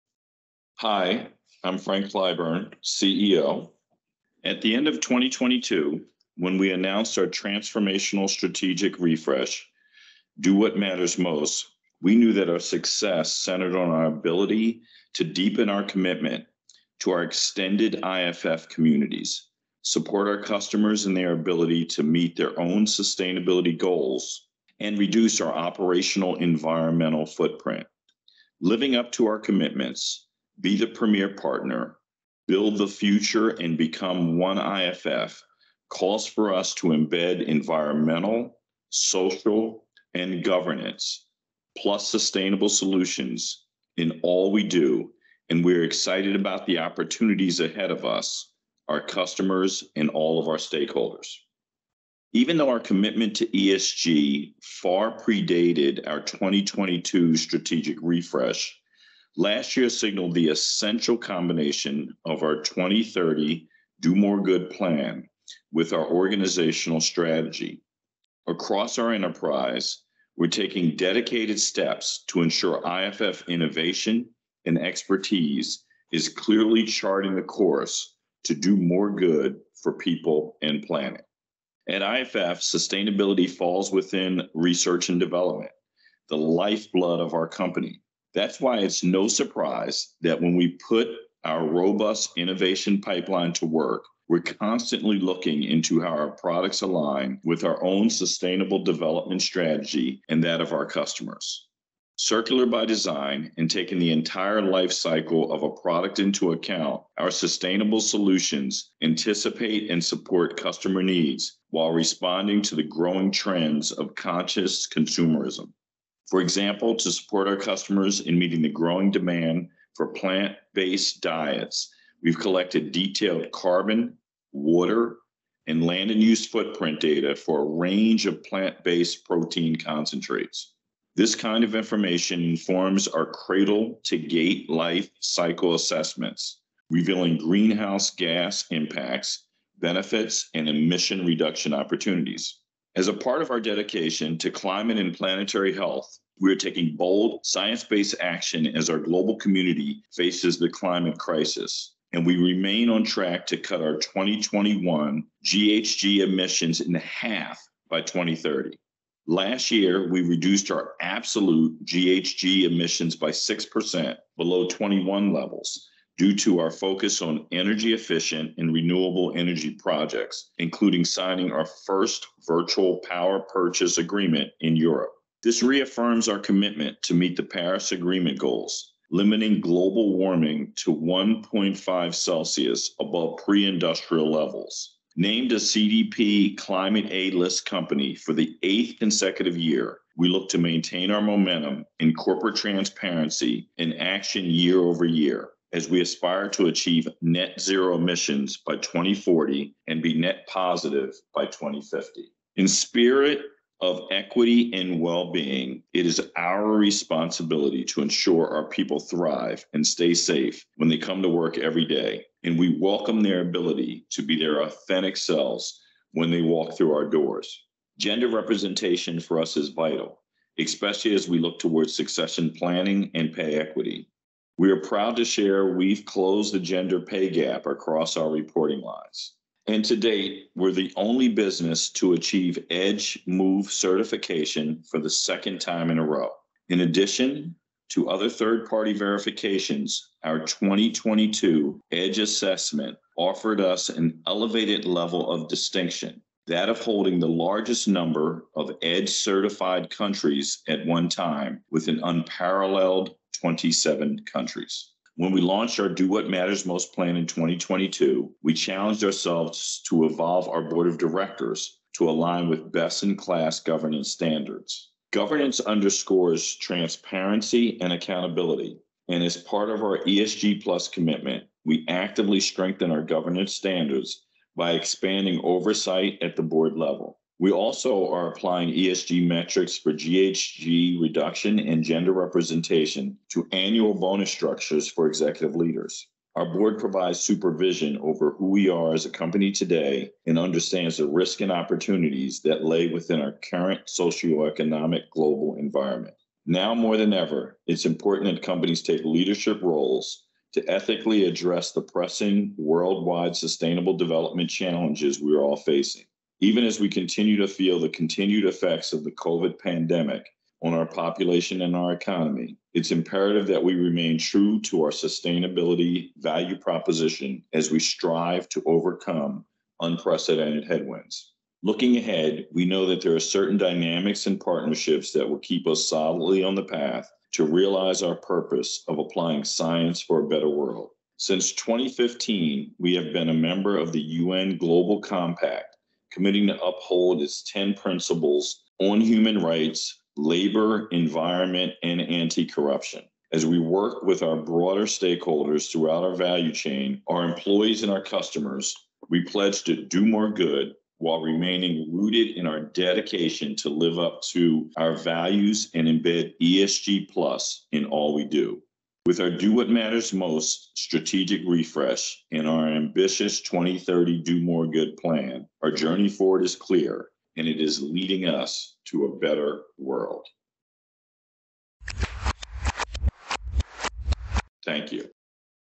Our 2022 ESG Report: Do More Good
Read our 2022 ESG Report to learn more about how we are applying science and creativity for a better world.